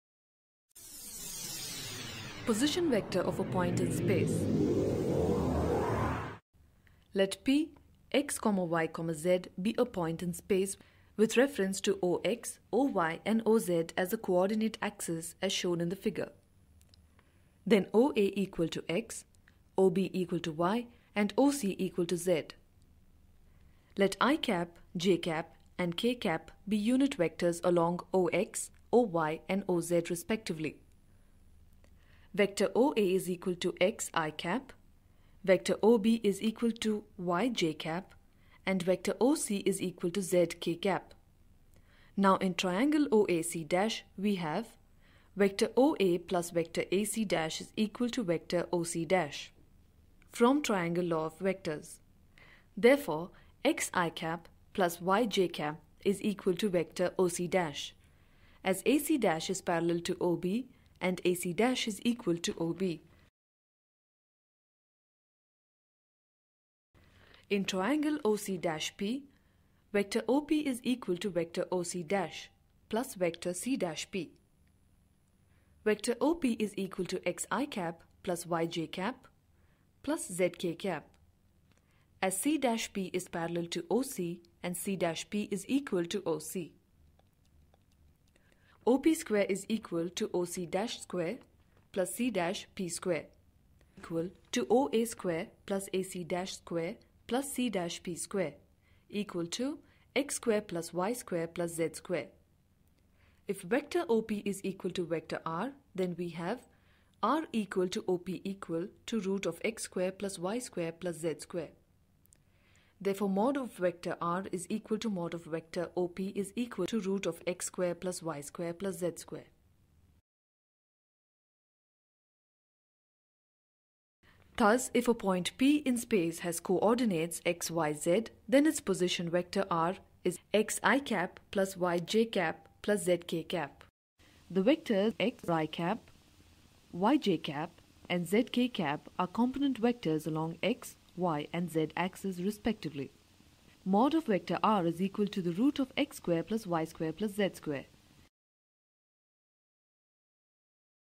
Position Vector Of Point In Space
Position Vector

A position vector is a vector that represents the position of the point P in space in relation to random suggestion origin O. Position vector is equivalent to the imaginary displacement from the point O to P. this type of concept is applied in 2 or 3 dimension space. It is also used as the deriving displacement by the comparison of 2 or more vectors.